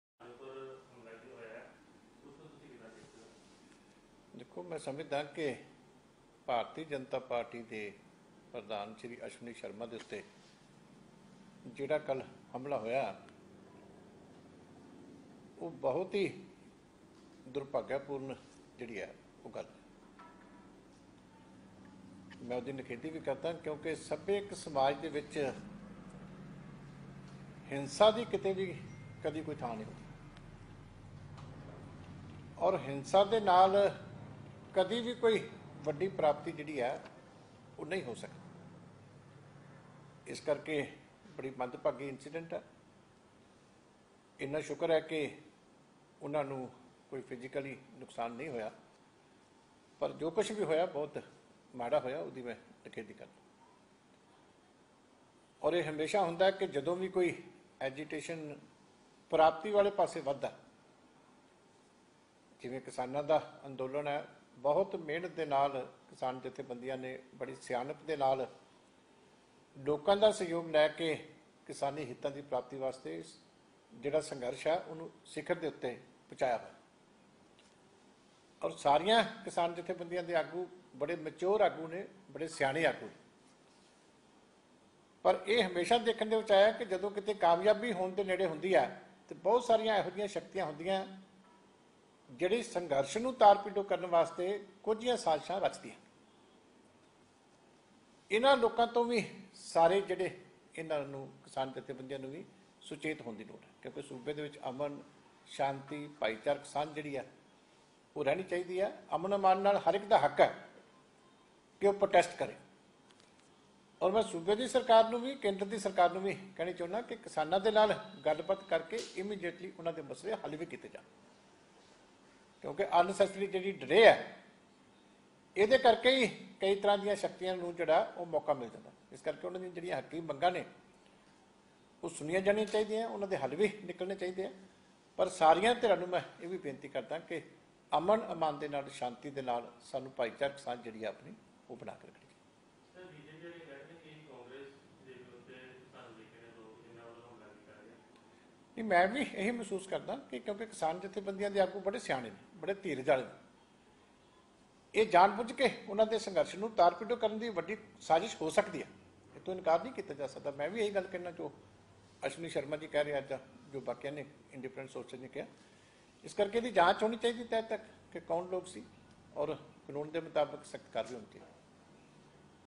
▶️ *भाजपा पंजाब प्रधान अश्विनी शर्मा पर हमले को लेकर अकाली दल का बड़ा बयान
▶️ *भाजपा पंजाब प्रधान अश्विनी शर्मा पर हमले को लेकर अकाली दल का बड़ा बयान, सुनो Dr. Daljit Singh Cheema ने क्या कहा.